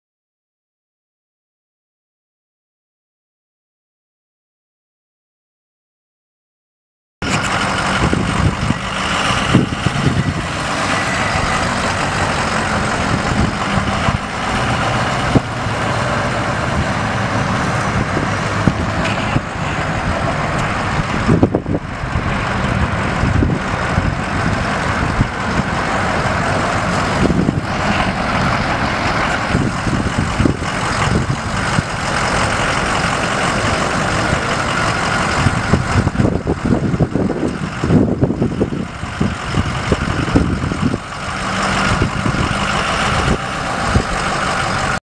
2006 Ford F250 Super Duty XL pickup truck for sale | sold at auction October 27, 2015
2006 Ford F250 Super Duty XL pickup truck
186,122 miles on odometer
6.0L V8 OHV 32V turbo diesel engine
Automatic transmission
AC and heat
Tilt
Cruise
AM/FM
Manual windows and locks
Headache rack
Cross bed toolbox
Fuel tank
Transfer pump
Front tow hooks
Receiver hitch
LT245/75R17 tires
1FTSF20P56EC33092